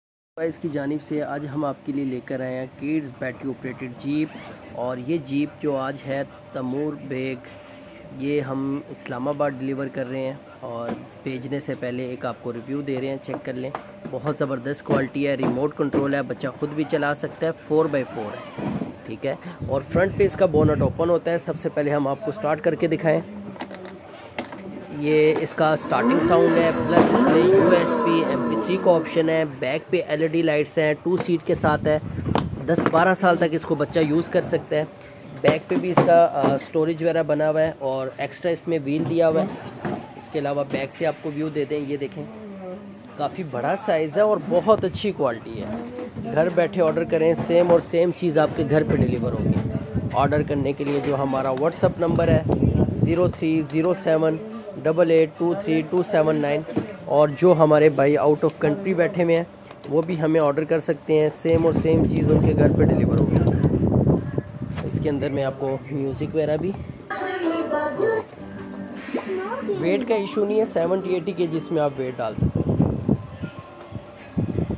Rc/938 speed kids battery operated jeep key start
Baby toys now launching all new  linced Model  big size jeep

Features:
🚙 MP3 player with SD card, AUX, USB port and BLUETOOTH connectivity
🚙  5powerful motors
🚙 Remote controlled
🚙 Self-driveable
🚙 Loads of leg room
🚙 LED headlights
🚙 LED taillights
🚙 Speed control
🚙 Seat belt
🚙 Smooth ride
🚙 Shiny rims
 🚙 Bluetooth
🚙 paintied body
🚙 color black white blue red
 Age limit: 2-10years old
Available colors: Red  blue white

Delivery available all across Pakistan! 🇵🇰